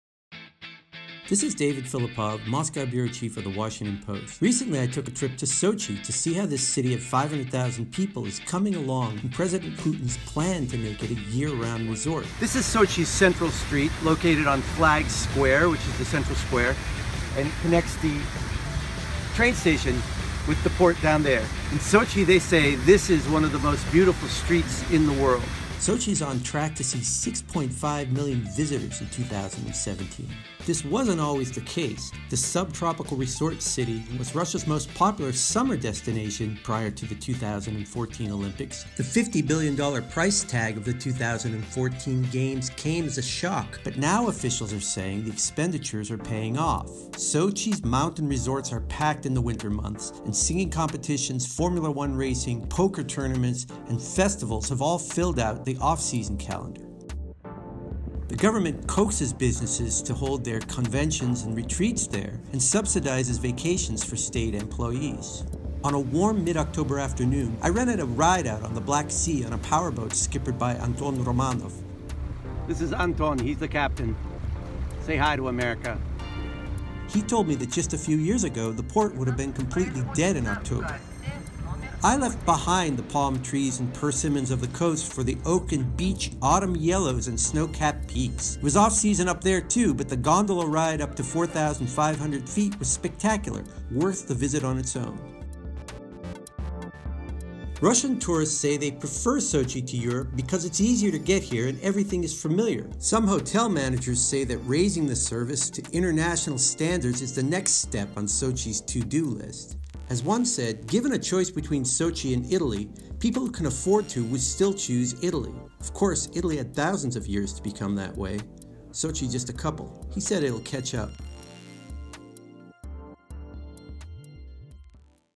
Four years after the Olympics, Sochi is an unlikely success story
The 2014 Winter Olympics’s $50 billion price tag was a shock, but Russian officials claim it’s paying off, as Sochi, traditionally a popular summer destination, now receives visitors all year round.